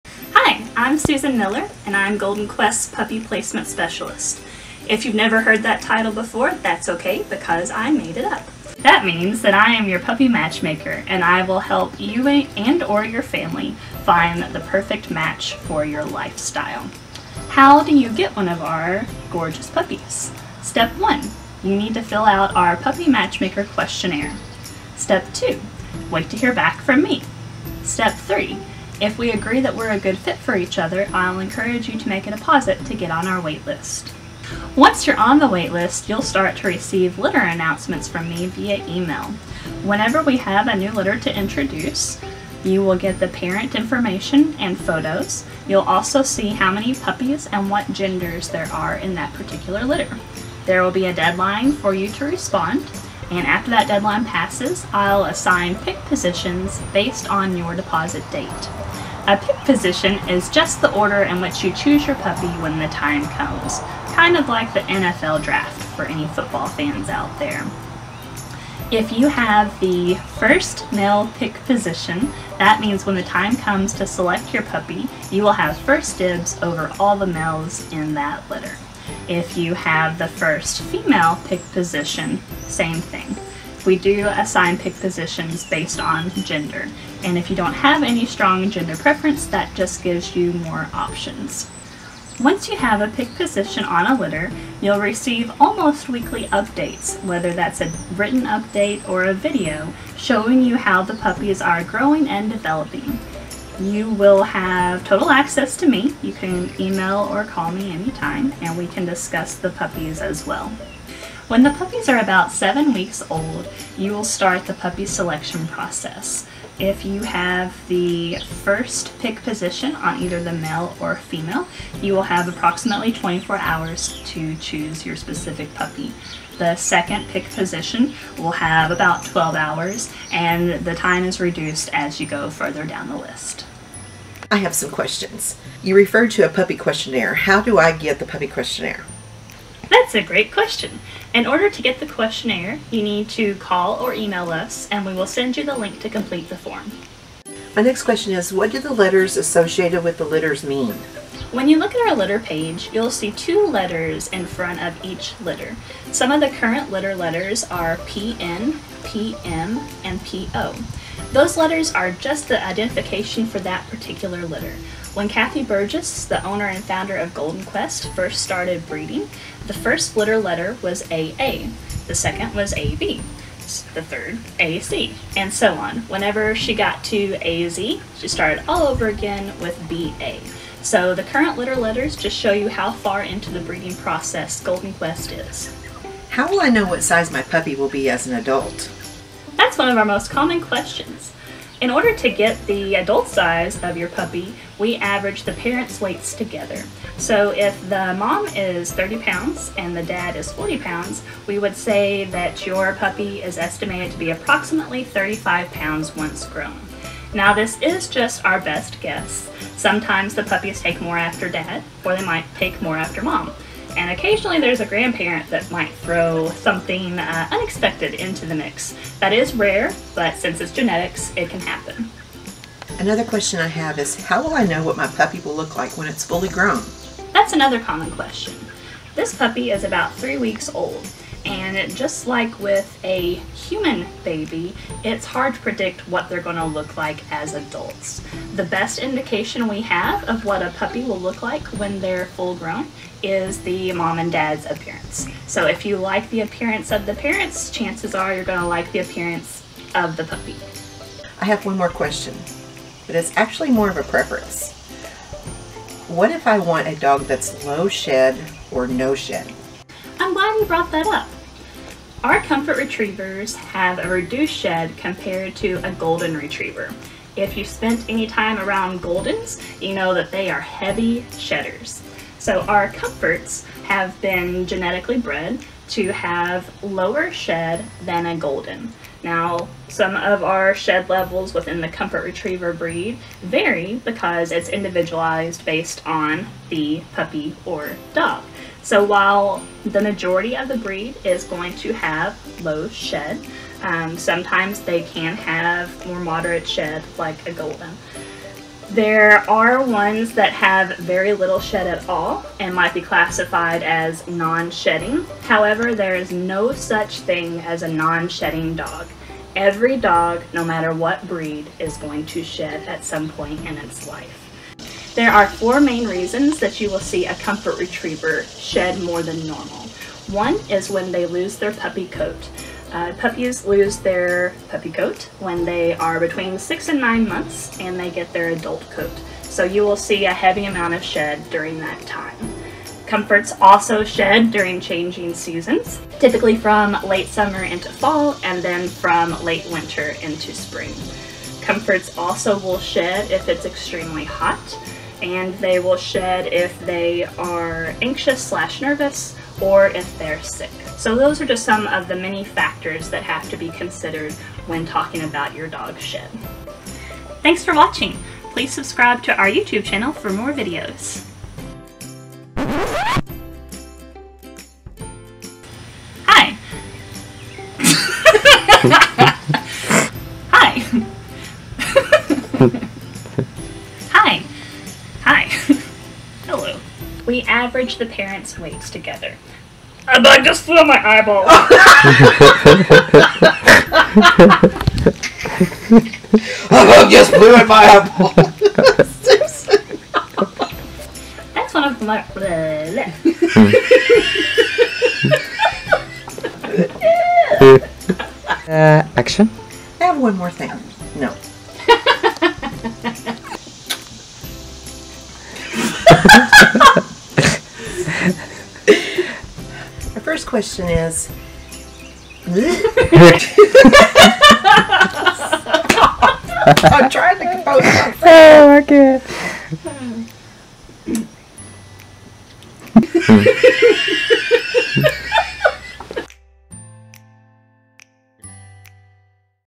About the Wait List and Pick Positions
In this video, Susan tells about our wait list and pick positions. She also answers some frequently asked questions.